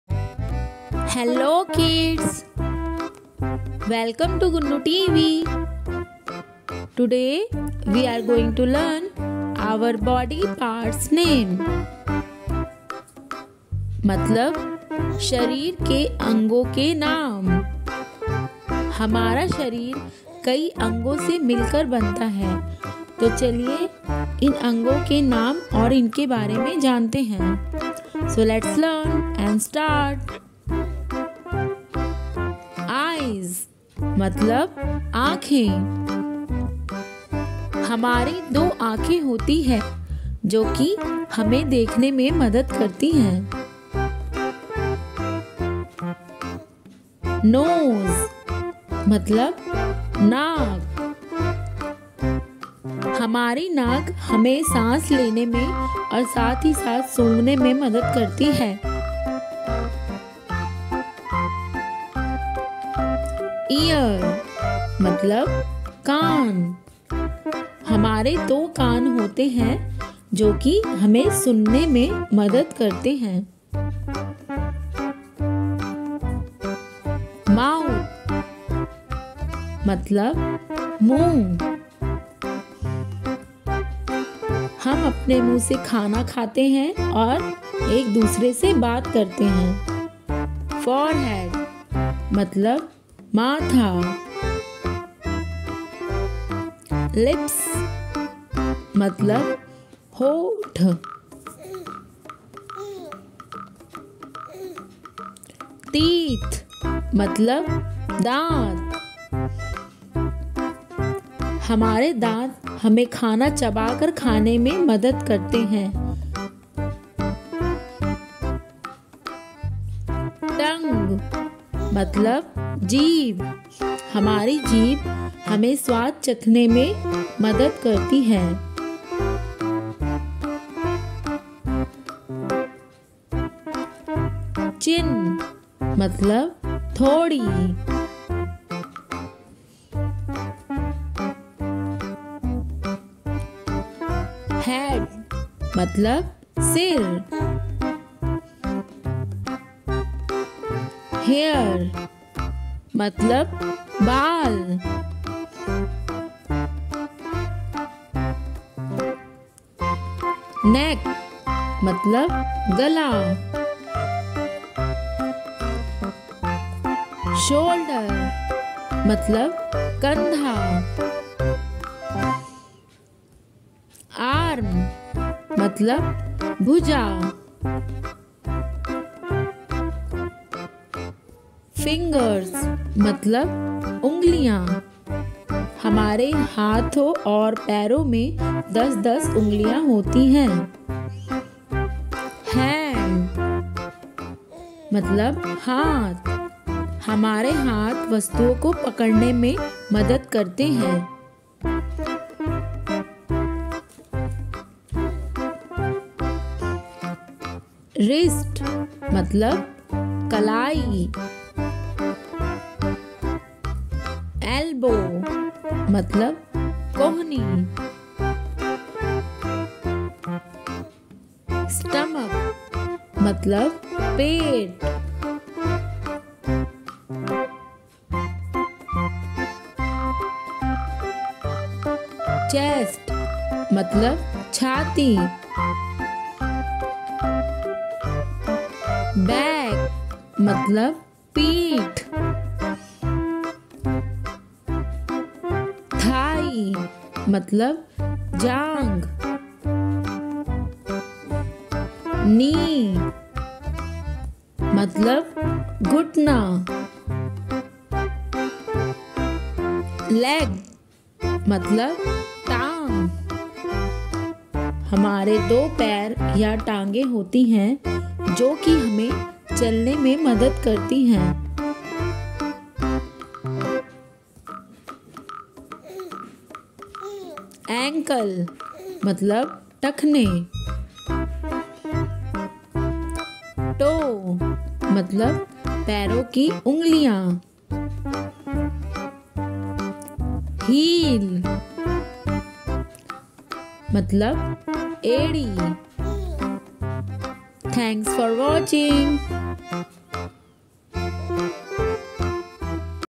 Body Parts name for kids | शरीर के अंगों का नाम | Human body parts name
Today we will learn about our body parts name. Many organs make a human body . So, let's learn their name and functions.